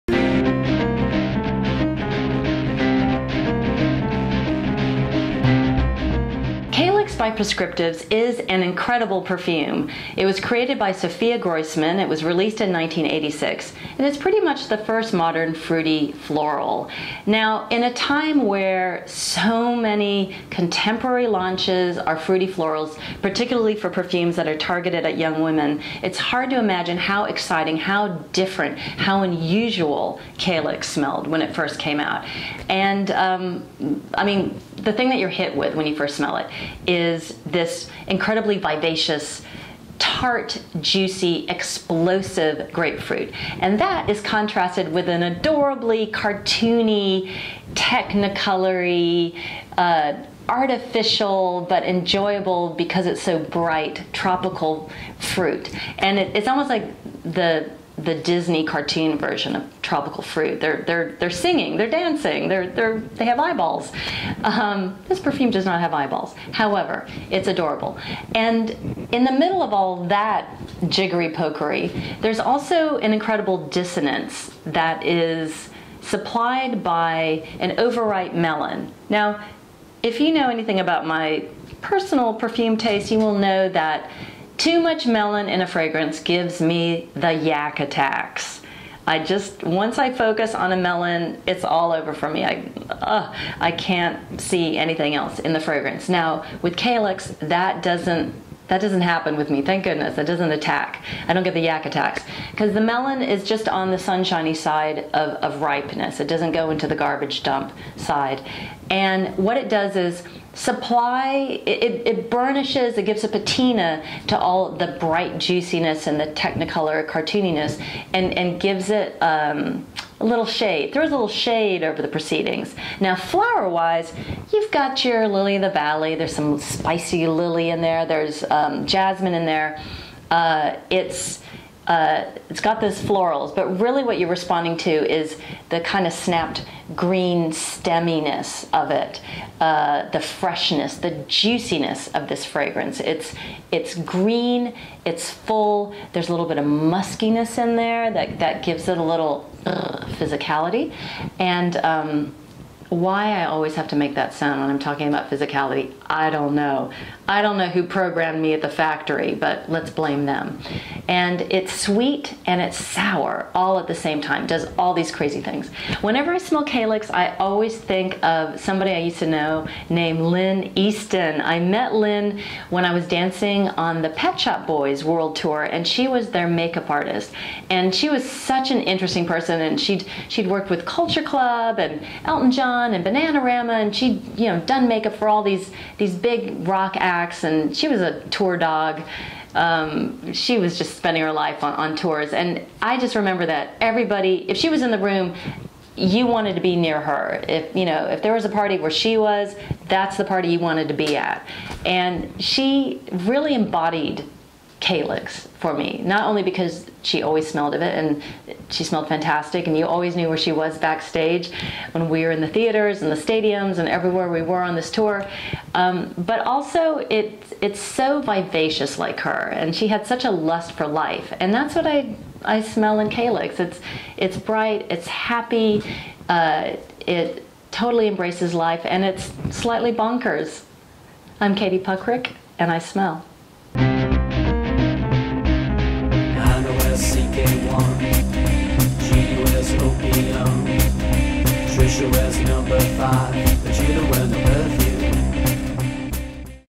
Calyx by Clinique: Perfume Review / Fragrance Review
Katie Puckrik smells Calyx, originally by Prescriptives, now sold under the Clinique brand. This review is dedicated to the memory of Lynne Easton, a vivid, creative, and adorable woman who wore Calyx with flair.

I first met Lynne when I danced on the Pet Shop Boys' 1991 "Performance" tour. Here's a clip from the show (I'm wearing a long, hot pink wig):

Looking for fragrance recommendations and gift ideas? Visit Fume Finder: the Katie Puckrik Smells fragrance app - it's free!